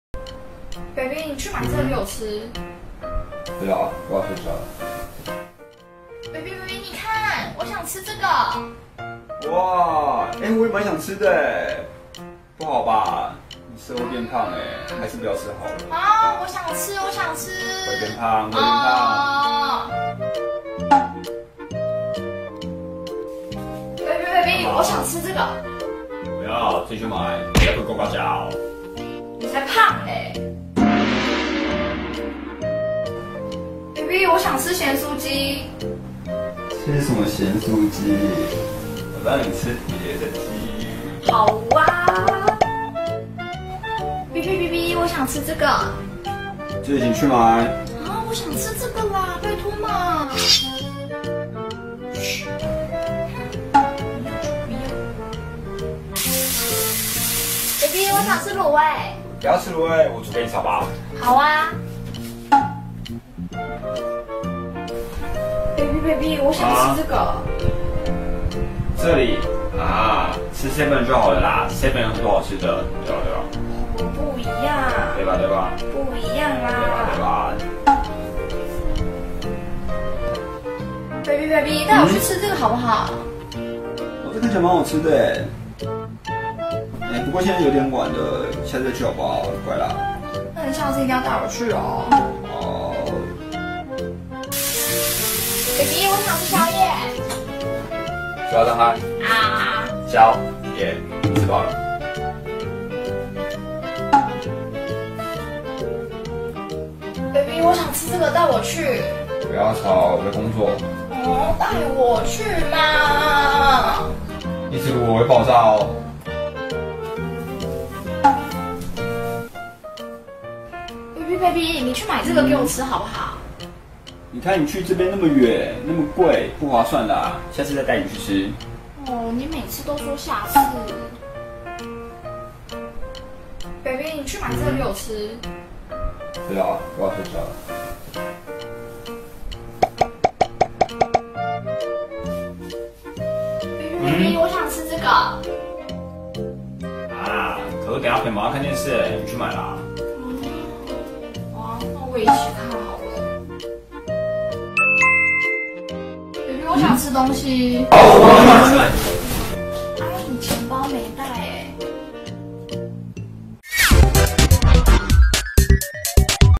當女友半夜肚子餓？！十二星座男友反應是？！《VS MEDIA》
說要睡覺的應該不只是雙魚座吧(拖別人下水的概念
不過熱戀期真的是不分星座都是一樣反應😂